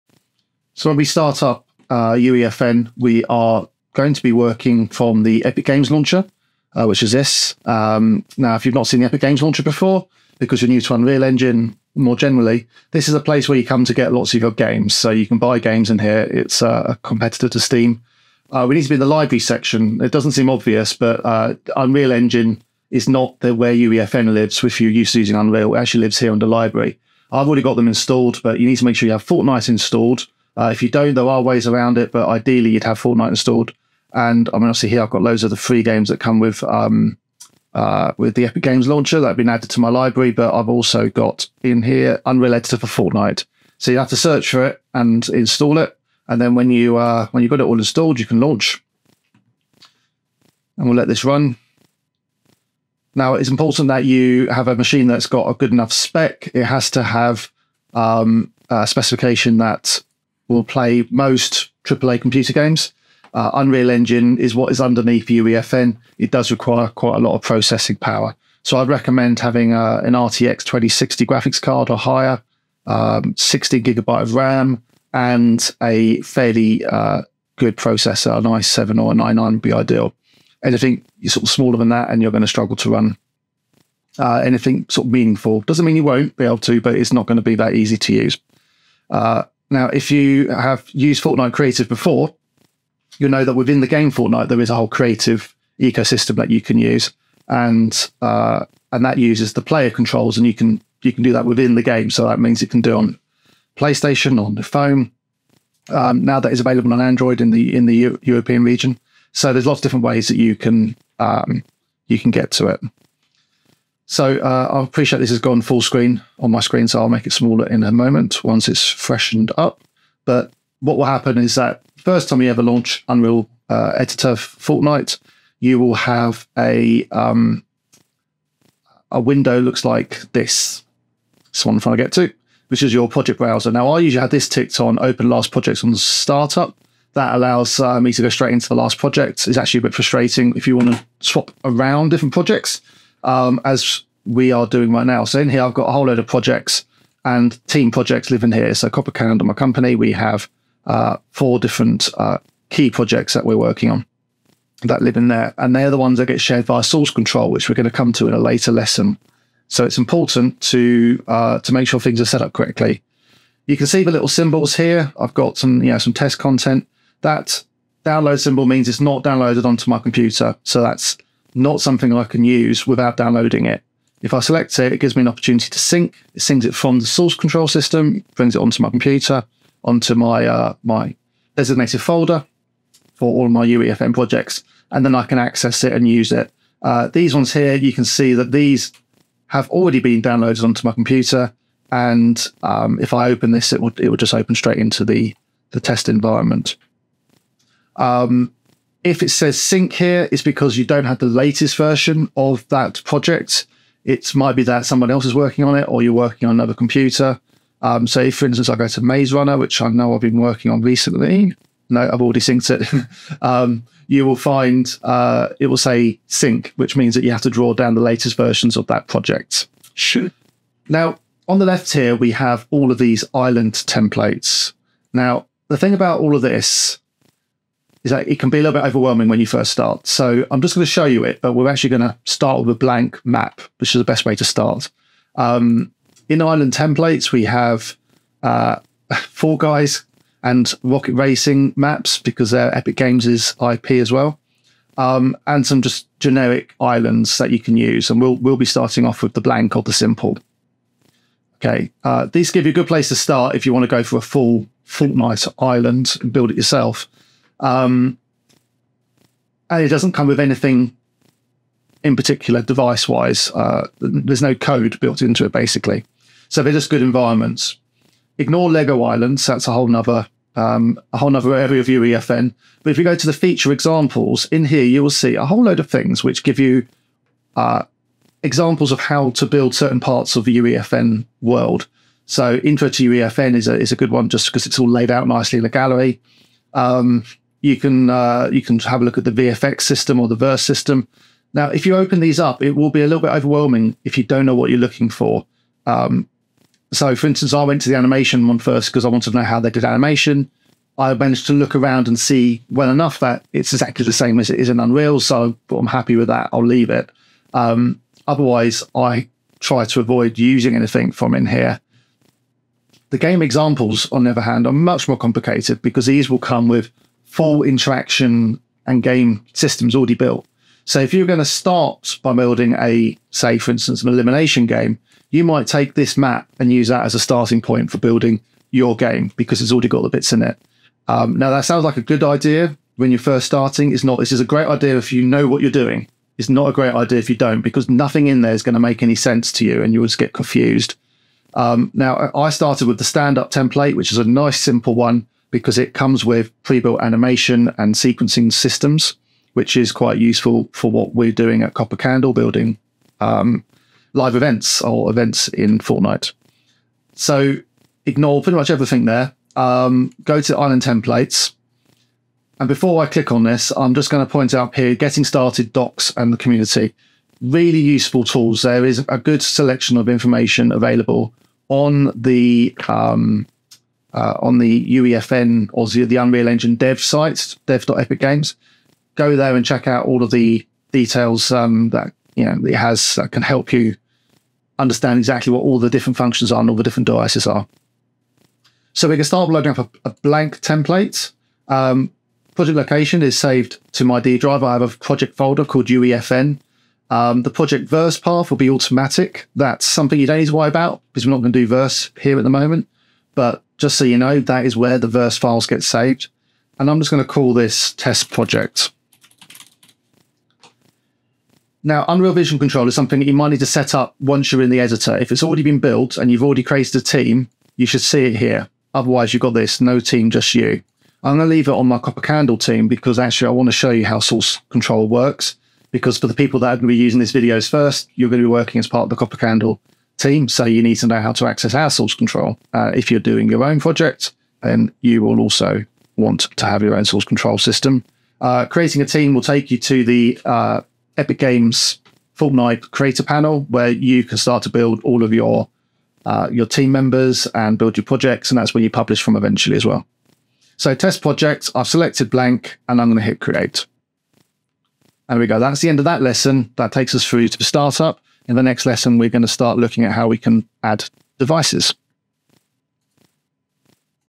UEFN Game Building for Beginners | Lesson 01 - Startup
Welcome to my 10-part series where I guide you through the basics of using Unreal Editor for Fortnite (UEFN) to build your very own Fortnite experience! Whether you're new to game development or just want to dip your toes into the world of UEFN, this series is perfect for you. I break down the complexities and show you how to avoid the need for coding with Verse, complicated devices, and hidden pitfalls. By the end of the series, you’ll have your own interactive game, ready to test and publish.

What you’ll learn in this series:

Introduction to UEFN and getting started
Placing actors and using Fortnite content and Epic assets
Source control basics
Creating player and guard interactions
Setting up simple devices and interactions
Working with characters and NPCs
Importing assets via Fab, Quixel, and Unreal Importer
Animation import and retargeting
Understanding validation and publishing your private test island
No prior gaming, coding, or development experience is required. Let’s get building!

Learn how to set up Unreal Editor for Fortnite (UEFN) from scratch. I’ll show you the basic interface, how to navigate, and what tools you’ll need to get your first project off the ground quickly and easily. By the end of this video, you’ll be ready to start building!